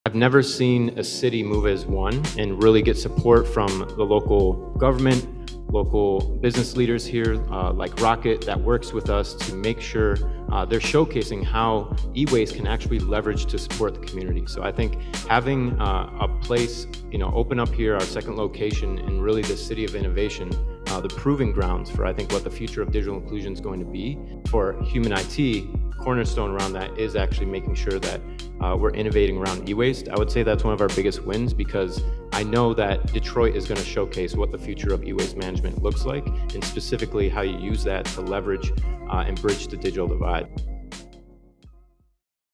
Detroit, the National Model for e-waste management
When it comes to leveraging e-waste to bridge the digital divide, no other city is like Detroit.

No other city has the winning trifecta of an innovative business community, an engaged local government, and a tenacious non-profit sector at its disposal to connect people in with the resources they need to thrive in the digital age.

Not yet, that is...